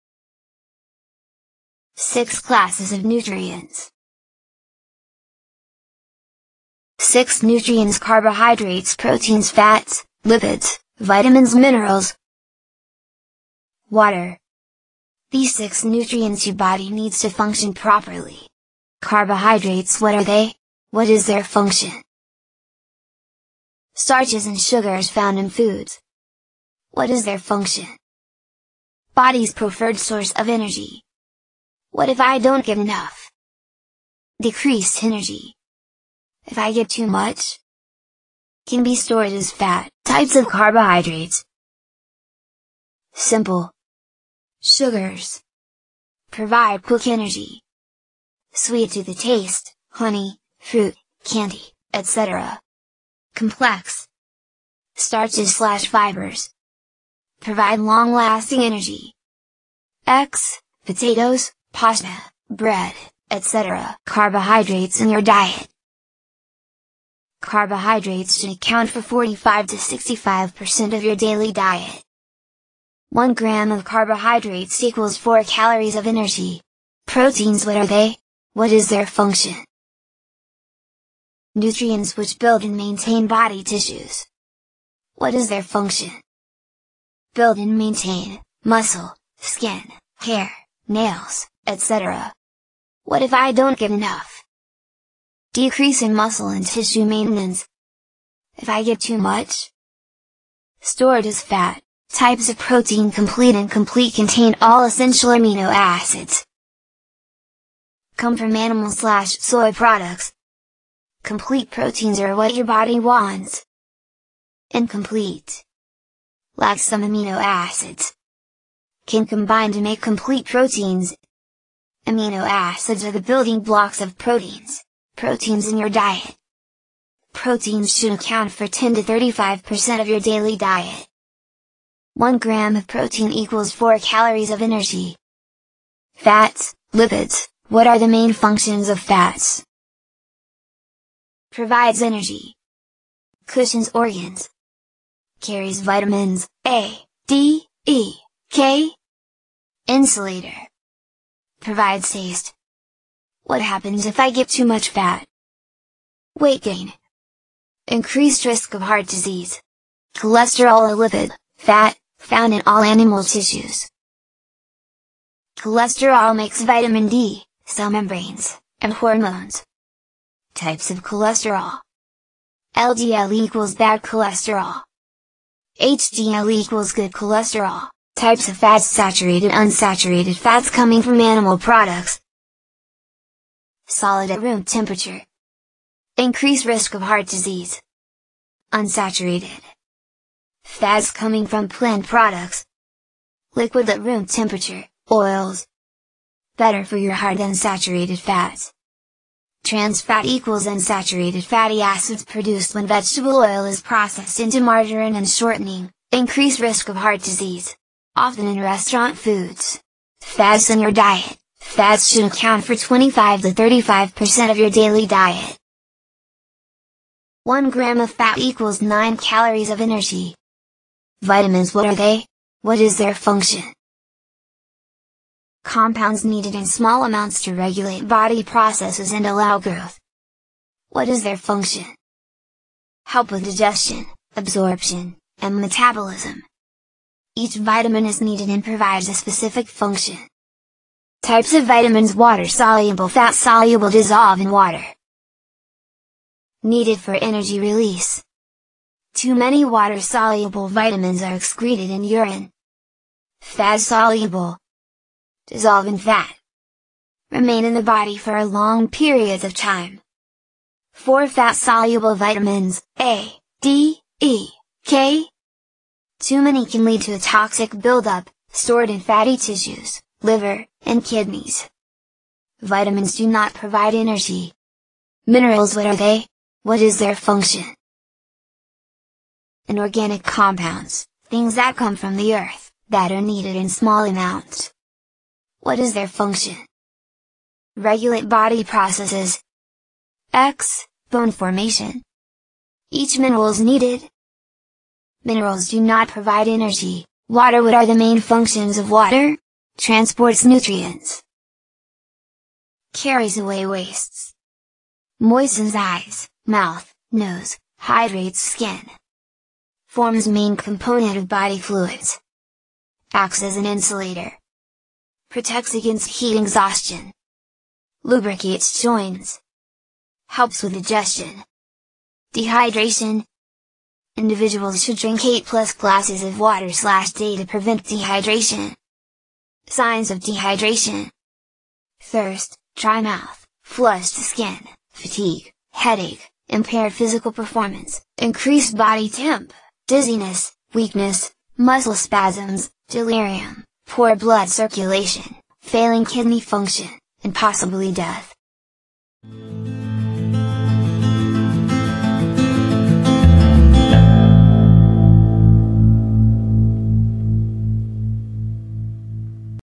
Nutrients and Their Functions - You Are What You Eat
Macronutrients are eaten in large amounts and include the primary building blocks of your diet — protein, carbohydrates, and fat — which provide your body with energy. Vitamins and minerals are micronutrients, and small doses go a long
6 kind of nutrients

1. mydoctors.knowabout.co.uk